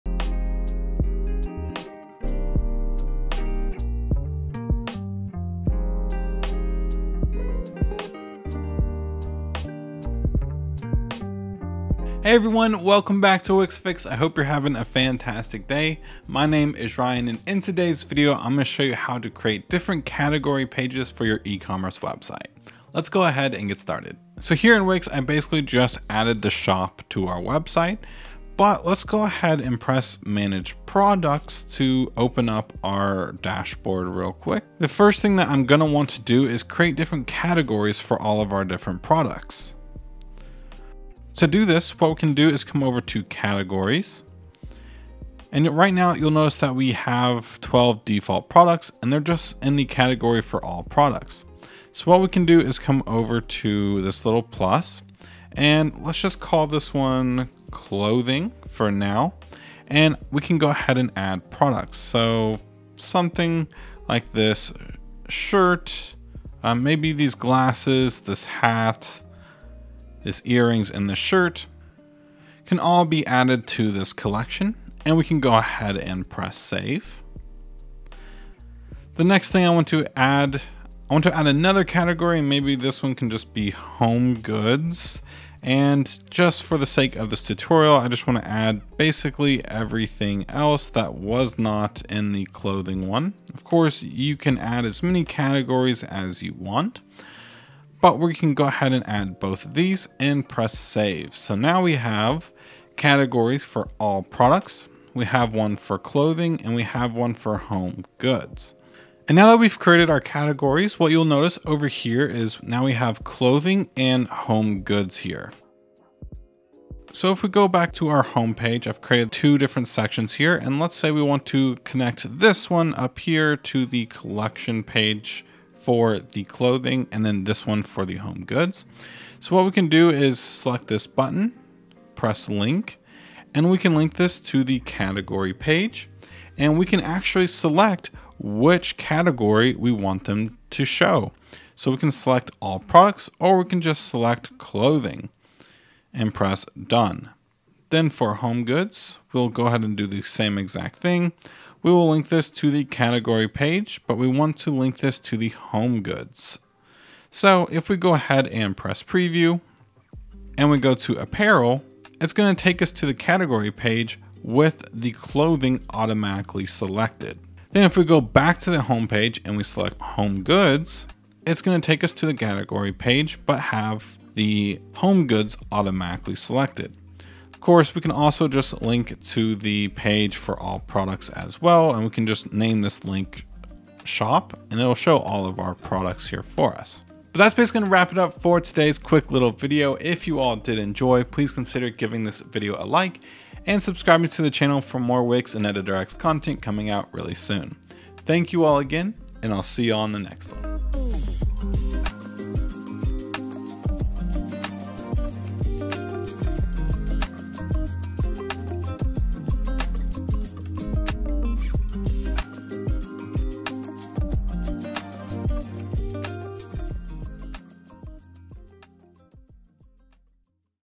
Creating Product Category Pages in Wix | Wix Fix
In today's ecommerce tutorial, you will learn the new method for creating category pages for your ecommerce products in Wix